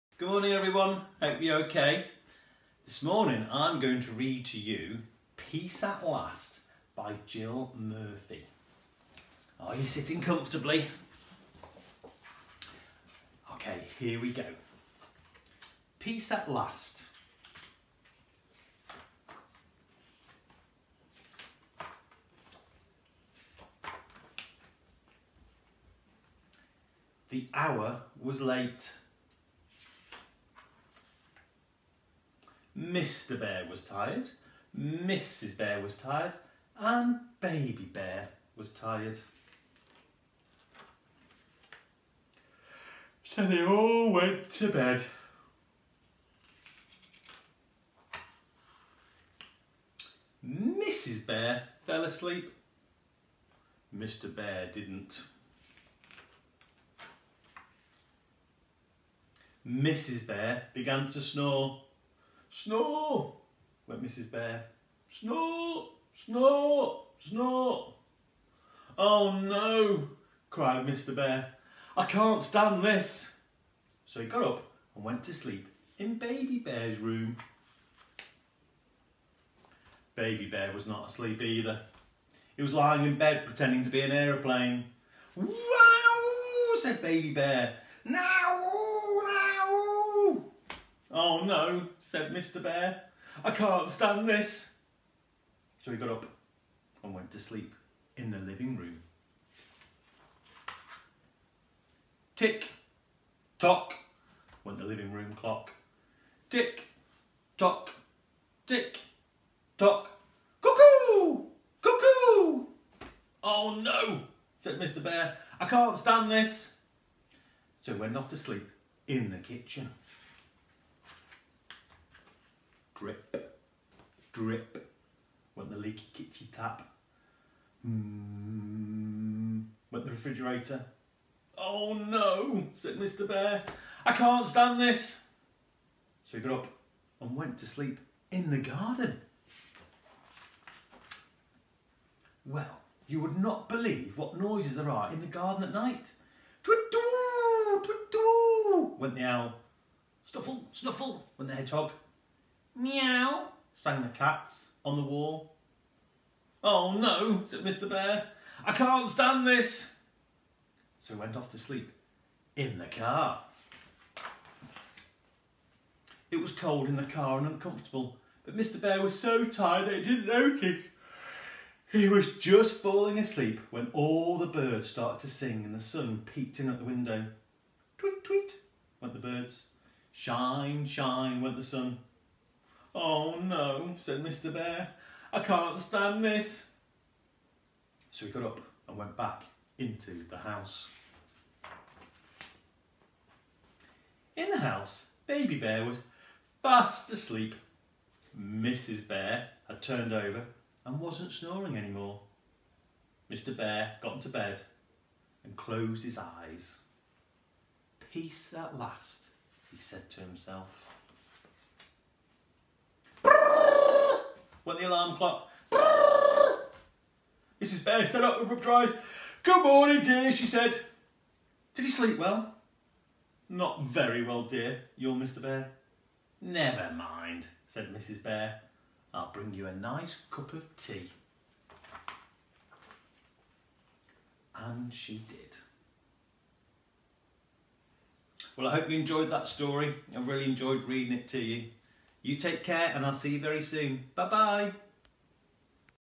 Mr. Clark reads 'Peace at last' by Jill Murphy
Mr. Clark reading 'Peace at last' by Jill Murphy for Reception and Year 1